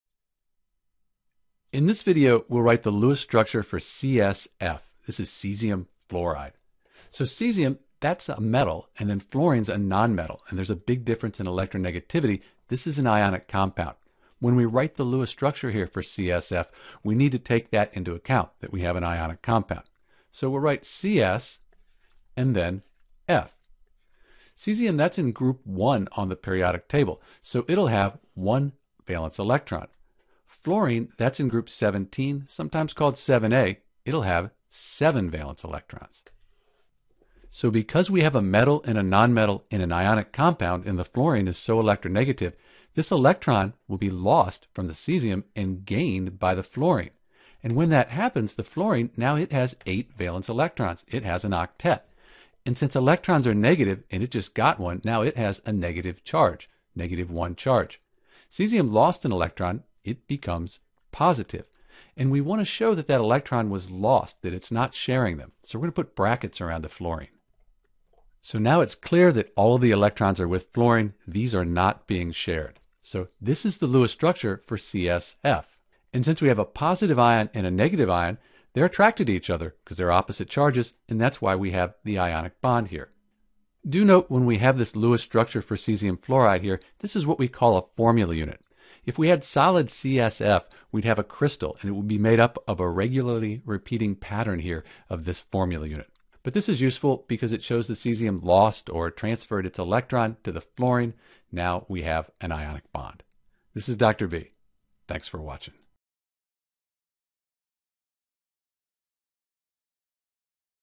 How to Draw the Lewis Dot Structure for CsF: Cesium fluoride
A step-by-step explanation of how to draw the CsF Lewis Dot Structure.

For CsF we have an ionic compound and we need to take that into account when we draw the Lewis Structure. We’ll first draw the metal and put it in brackets with its charge on the outside (video: finding ionic charge

Next, we'll draw the Lewis Structure for the F- ion and add brackets. We put the two ions together to complete the Lewis structure for CsF. Note that CsF is also called Cesium fluoride.

Note that is it more common to draw Lewis Structures for covalent (molecular) compounds where valance electrons are shared. In the case of ionic compounds, where we have a metal bonded to a non-metal (or group of non-metals), the Lewis diagram represents a formula unit. Many of these formula units make up a crystal lattice. So when we talk about the structure for CsF we think of it together with other CsF formula units in a crystal (NaCl is a good example of this